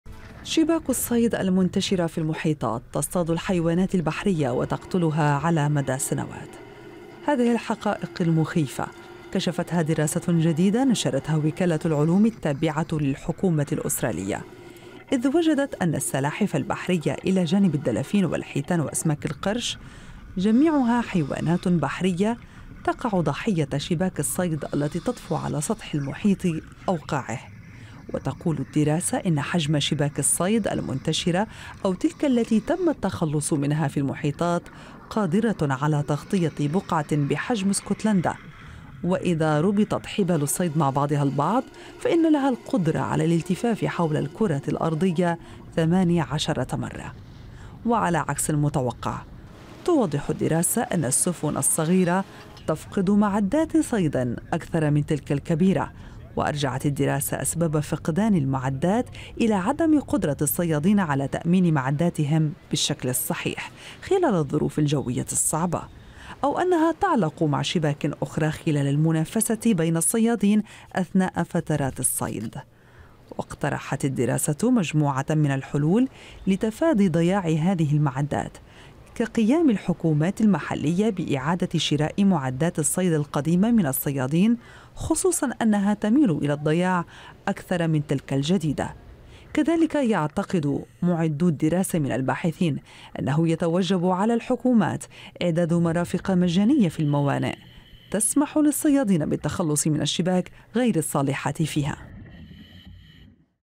مخلفات شباك الصيد المنتشرة في المحيطات تقتل الحيوانات على مدى سنوات
نسلط الضوء على الخطر الكبير الذي تشكله شباك الصيد المنتشرة في المحيطات على الحيوانات المائية